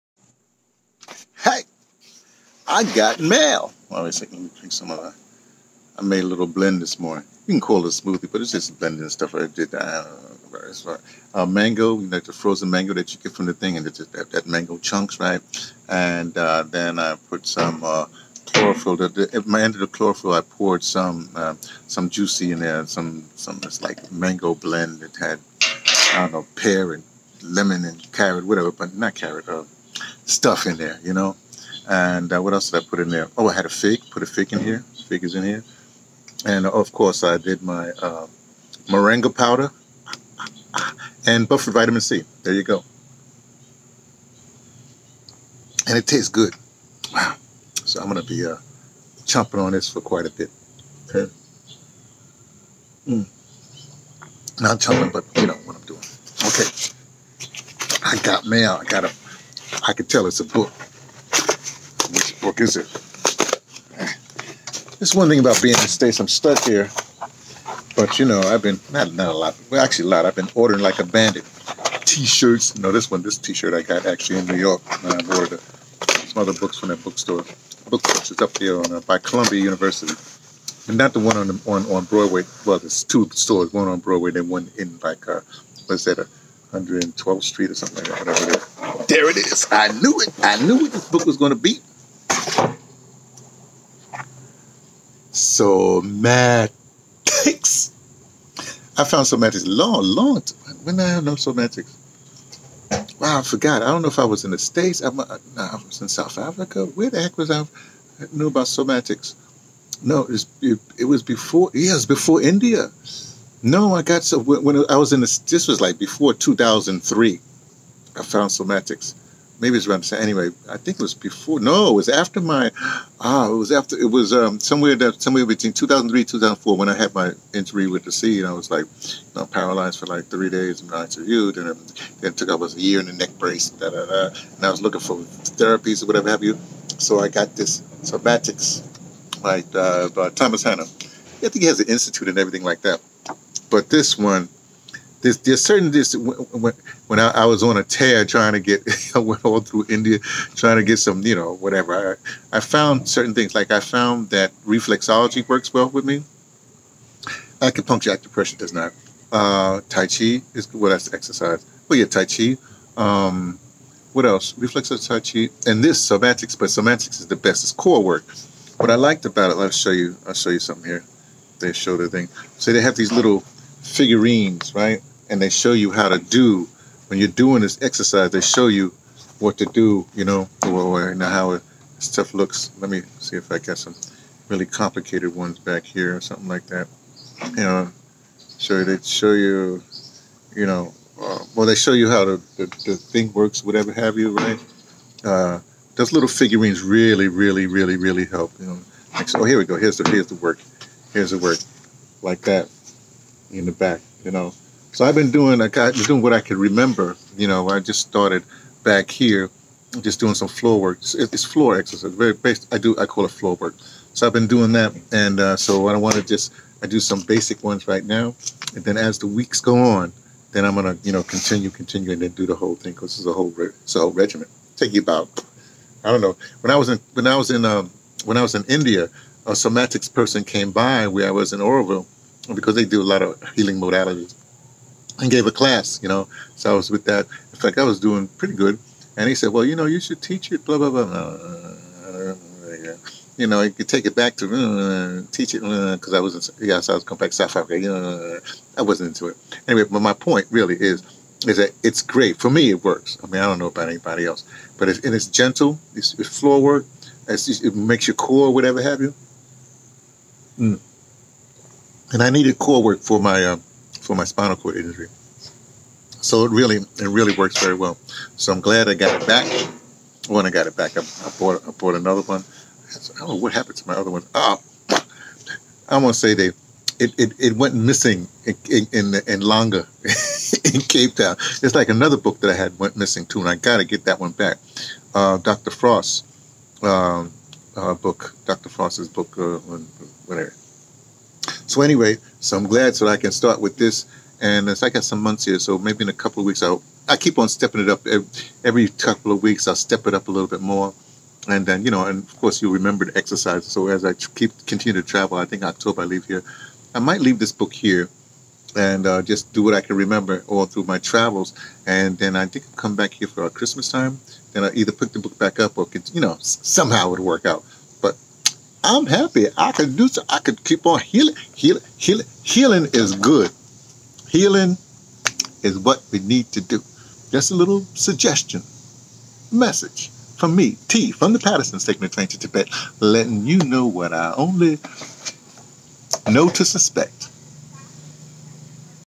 For Floor (core) Work
One of the only healing modalities to work on DaBrother's battle scared body is Thomas Hanna's SOMATICS: reawakening the mind's control of movement, flexibility, and health.
It came as a delivery; right on time as he intensifies his morning work-out routine(s).

Crypto Currency /BitCoin CONTRIBUTIONS:
722fce22-5e9f-42fc-a378-2da90f5d267a